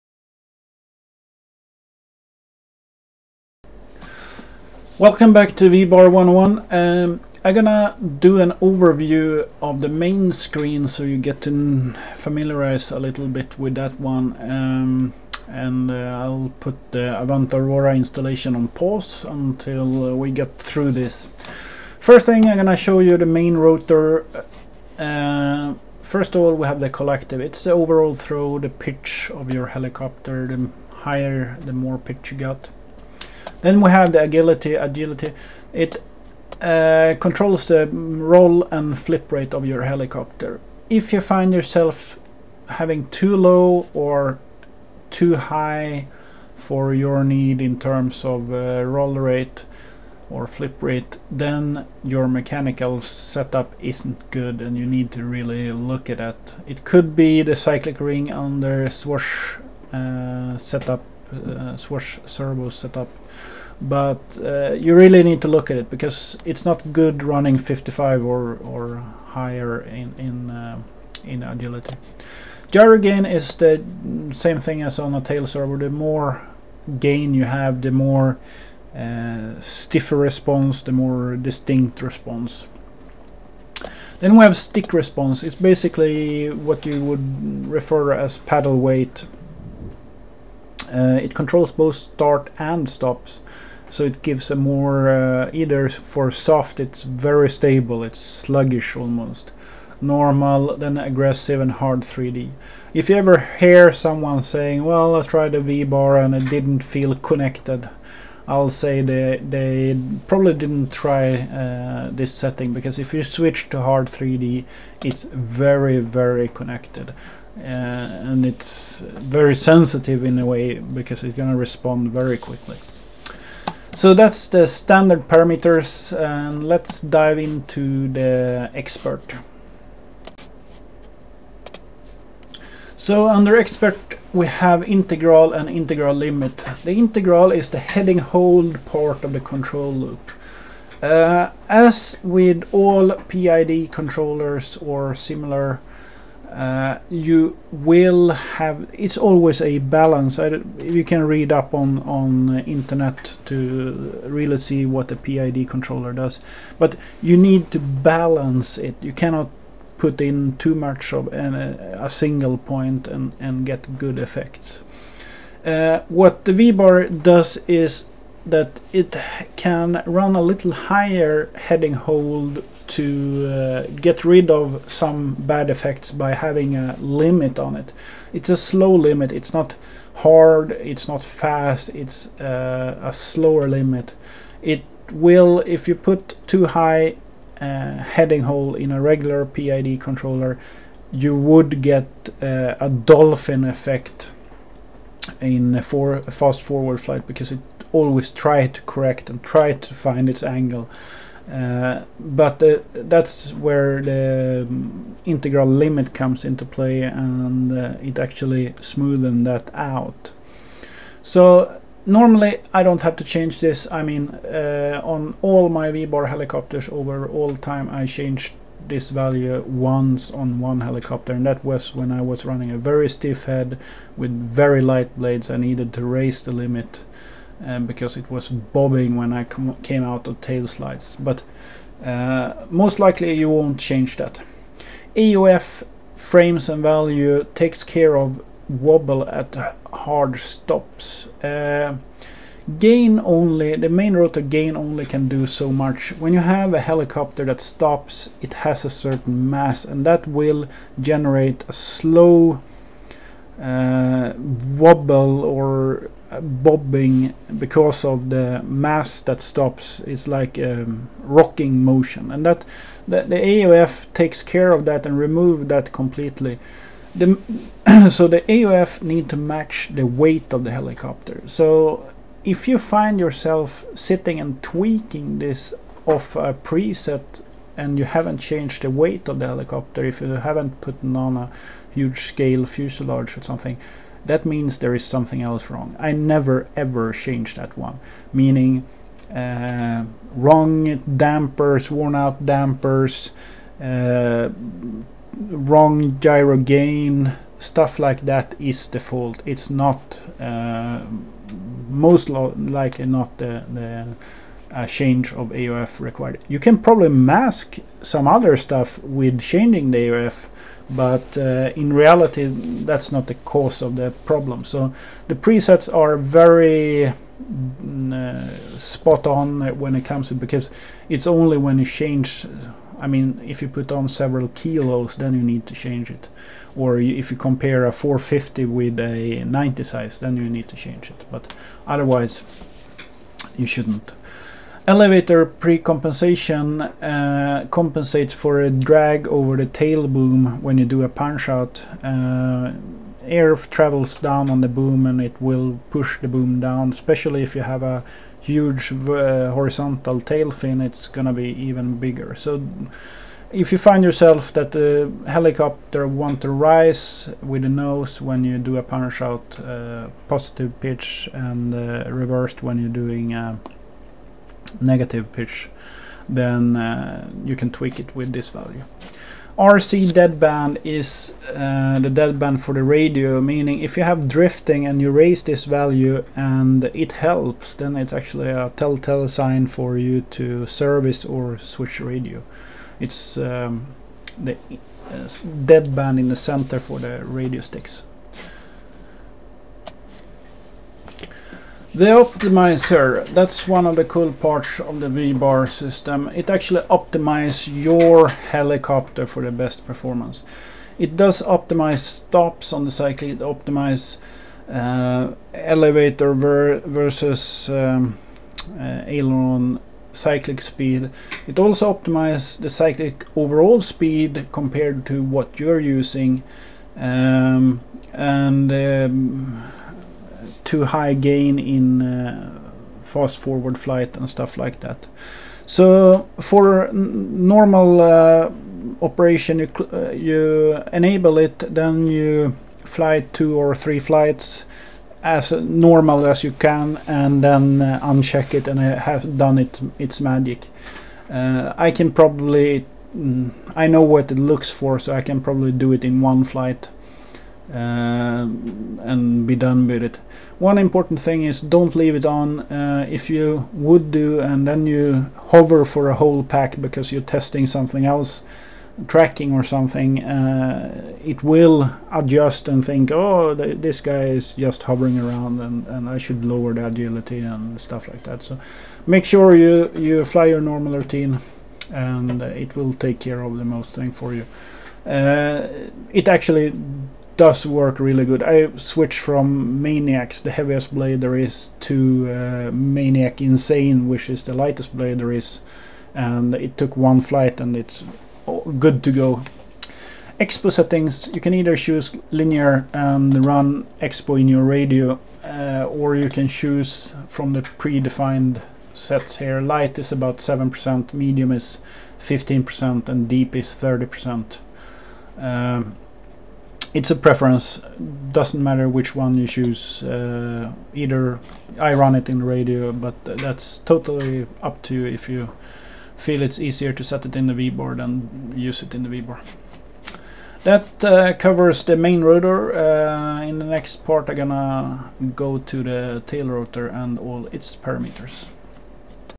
VBar 101 - Episode 13 - Main Screen - Main Rotor
Episode 13, I have paused the Avant Aurora setup to make an overview of the mainscreen so you get a deeper feeling of all the parameter.

This a lot of info in these sessions ;)